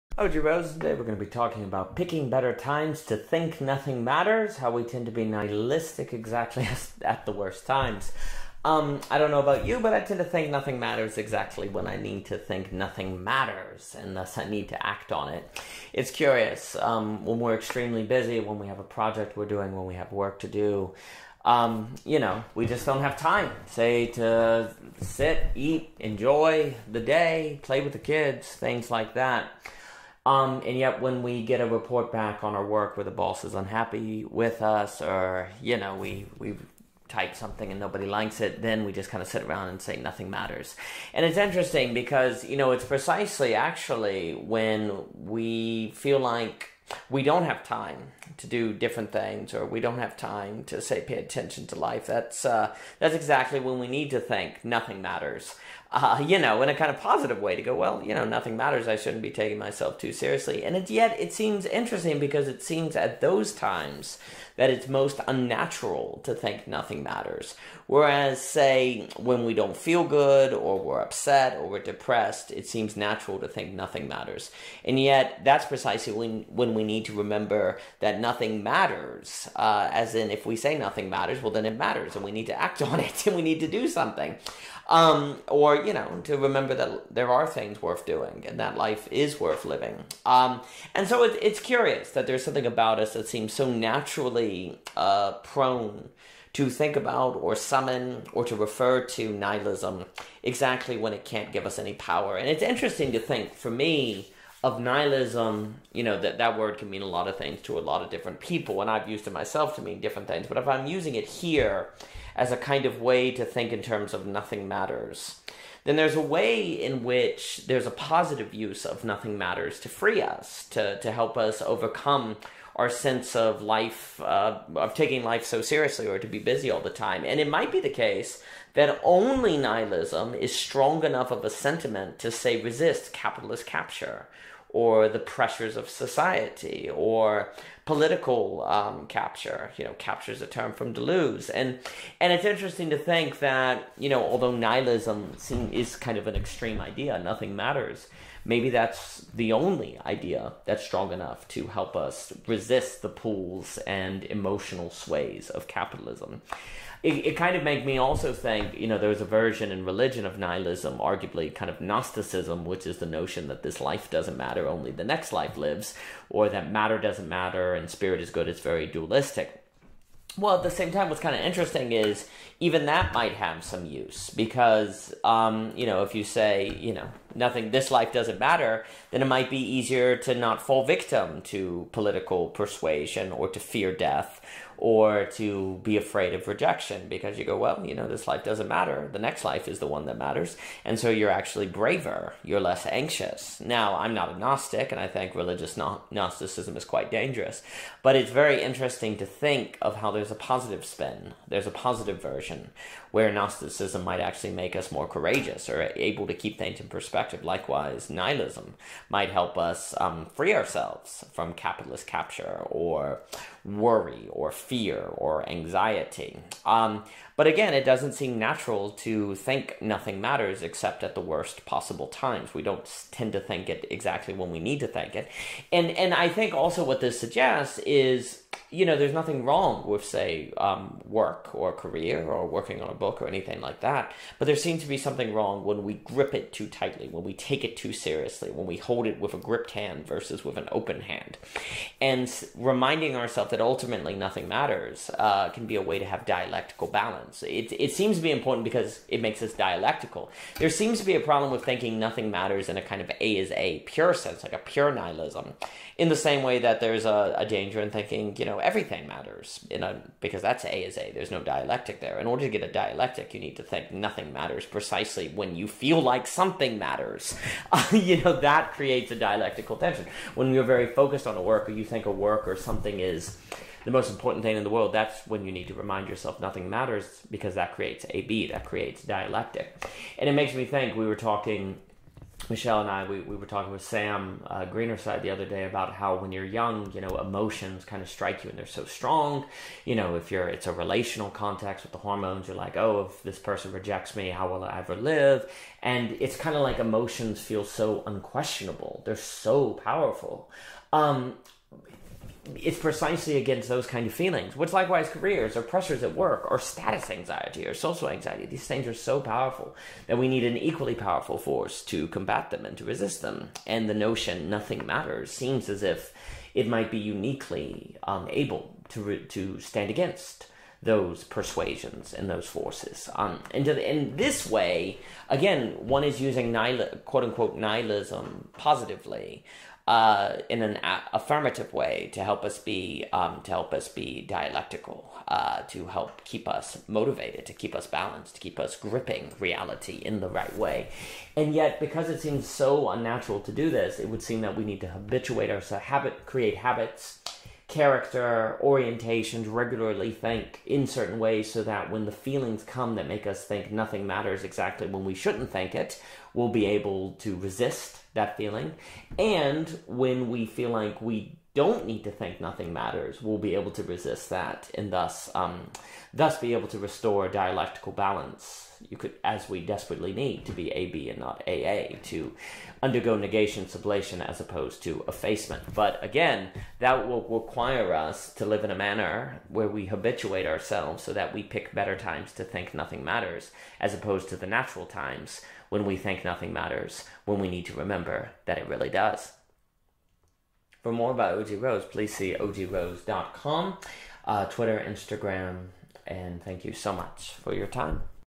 Picking Better Times to Think “Nothing Matters” by O.G. Rose
It’s funny, but I think we tend to think “nothing matters” exactly when we need to think “nothing matters” (and thus I need to act on it), as we think “nothing matters” precisely when we need to slow down and pay attention to something else. Why is it that when our kids want to play that we’re busy, but when we want to watch Netflix we suddenly have time? There’s something strange about humans in this way—we’re not very good at sizing up our own situations—and I can’t help but think that it would be good to remember that “nothing matters” when my kids want to play. I do have time. Nothing matters.

Nihilism can be useful to help us avoid “taking ourselves too seriously” (even if the most fundamental forms of nihilism are impossible, as argued in Missing Axioms by Samuel Barnes). Yes, nihilism can go too far, but there is also something about it that can help us “hold things looser.” There’s nothing wrong with working hard, but if that work is keeping us from everything else in our lives, it might actually be good for us to remember that it ultimately “doesn’t matter.” From what I can tell, this is a realization that a lot of people suddenly have on their deathbeds: what they thought was important wasn’t really that important, and what they didn’t think they had time for was what they should have spent more of their time on. But I also can’t help but assume that nobody intends to make this mistake, and probably a lot of people work hard to avoid it, and yet countless people make the mistake all the same. How? Well, maybe it’s because work at the time really feels like we need to focus on it, and we don’t feel like we have time for playing hide-and-seek. We’re not being arbitrary and careless when we tell our kids we’re busy—I mean, we are busy, right? And work is only demanding more of us these days—bills to pay, inflation worsening—on and on. The pressures and pulls are strong and real, and yet this might be precisely why we need nihilism. Nihilism might have enough strength to fight against these real and powerful tensions...

Photo by Denise Johnson